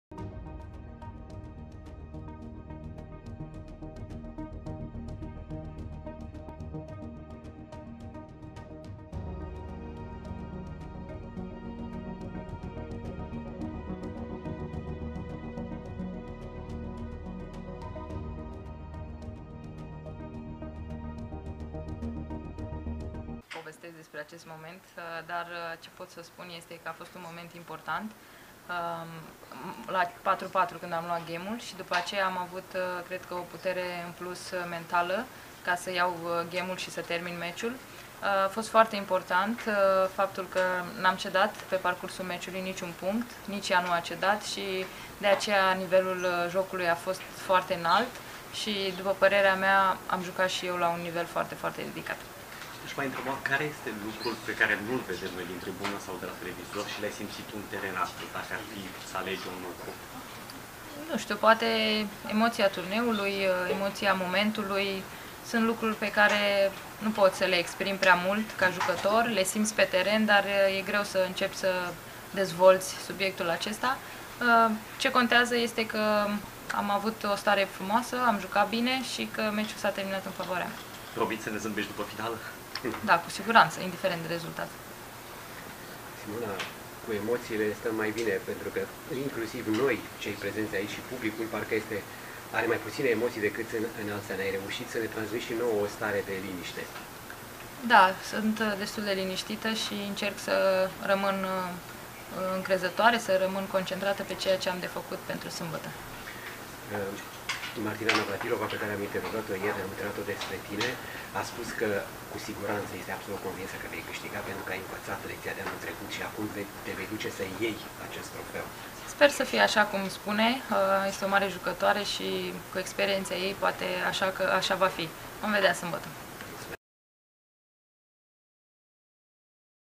Halep despre momente dificile. "In finala o sa fiu cu zambetul pe buze"
Simona Halep inainte de finala cu Sloane Stephens . Are joc mult mai relaxat. dar nu e o tragedie daca pierde. Asta e atitudinea buna, dr acum in afara de aceasta aborsdare a declarat cavrea titlul.